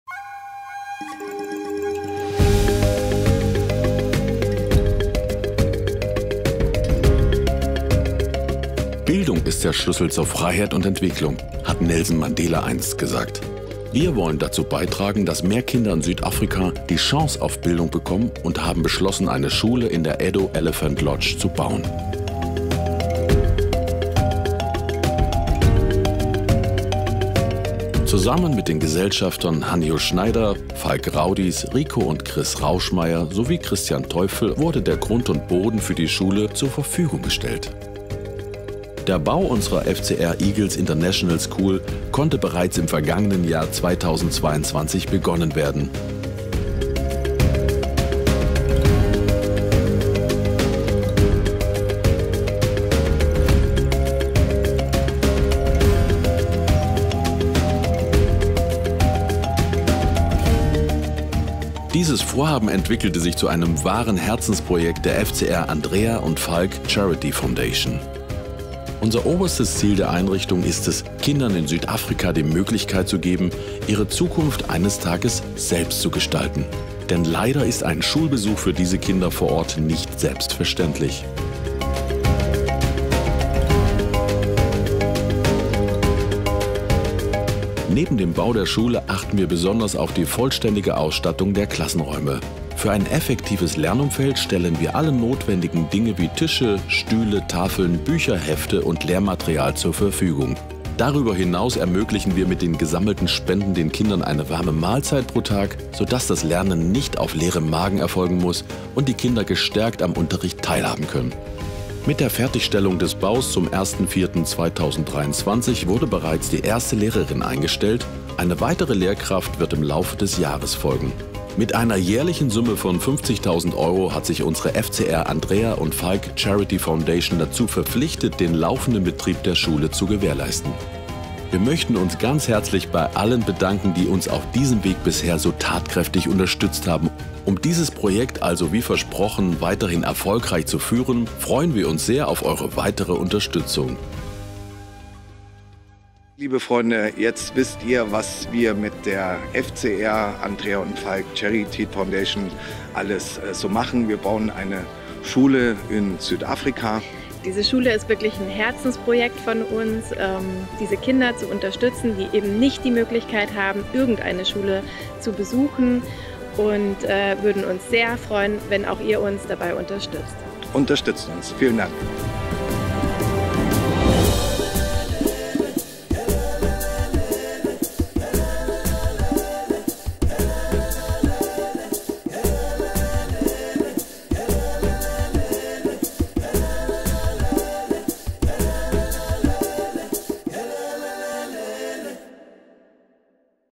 FCR EAGLES INTERNATIONAL SCHOOL | Share your best moments
Ein Herzensprojekt in Afrika.

Mehr Kinder sollen die Chance auf Bildung bekommen! Und das haben Falk Raudies, Hanjo Schneider, Ricoh & Chris Rauschmayer und Christian Teufel umgesetzt.

Grund und Boden wurden für die Schule zur Verfügung gestellt - der Bau der FCR International Eagles School startete bereits 2022.

Ganz besonders ins Herz getroffen hat es Andrea & Falk Raudies. Mit ihrer FCR Andrea & Falk Charity Foundation.

OBERSTES ZIEL DER EINRICHTUNG:
KINDERN IN SÜDAFRIKA DIE MÖGLICHKEIT BIETEN IHRE ZUKUNFT SELBST ZU GESTALTEN.

Und so werden Klassenzimmer ausgestattet, Lehrer eingestellt, Lehrmaterial gekauft, Mahlzeiten ausgegeben. Hier muss keiner hungrig in den Unterricht.

Wir sagen 🔝 zu diesem Projekt.